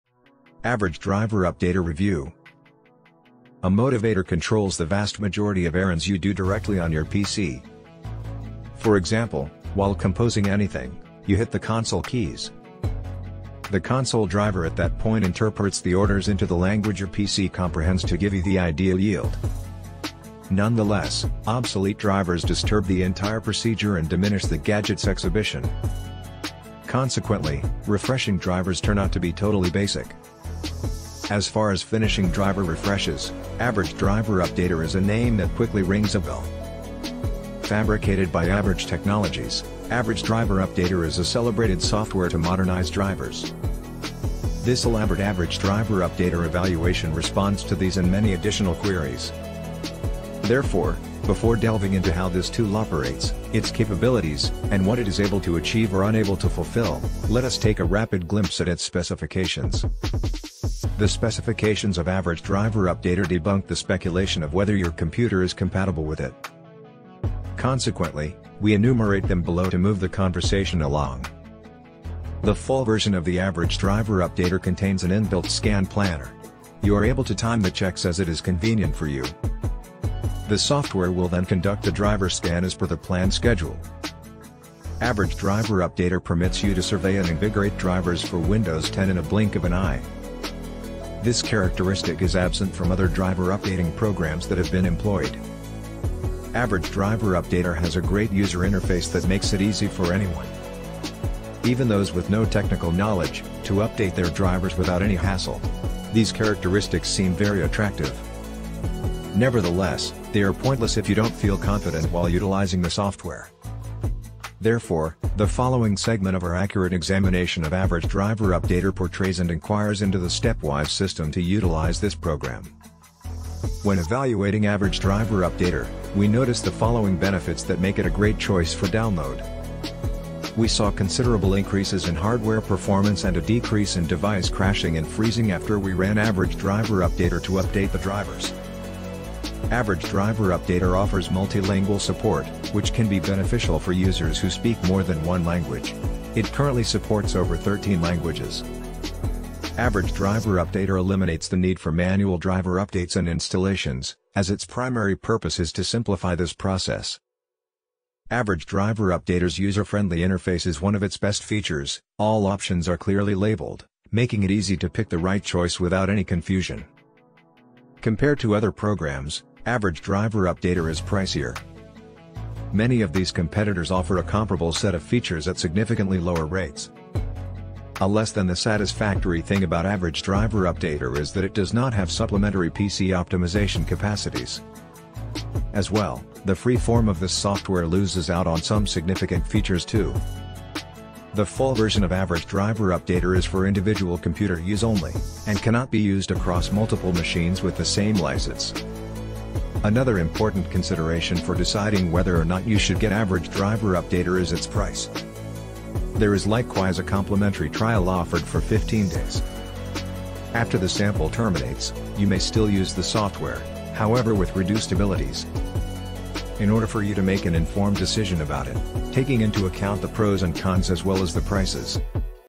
AVG Driver Updater Review | Comprehensive Updater
In this video review, we take a closer look at AVG Driver Updater, a software designed to help users keep their drivers up-to-date and their PC running smoothly. We explore its features, including its automatic scanning and updating of outdated or missing drivers, its backup and restore functionality, and its scheduling options. We also examine its user-friendliness, design, and pricing. Additionally, we discuss its performance and effectiveness in improving system stability and performance. Whether you're a novice or experienced PC user, this review will give you a comprehensive overview of AVG Driver Updater and its capabilities.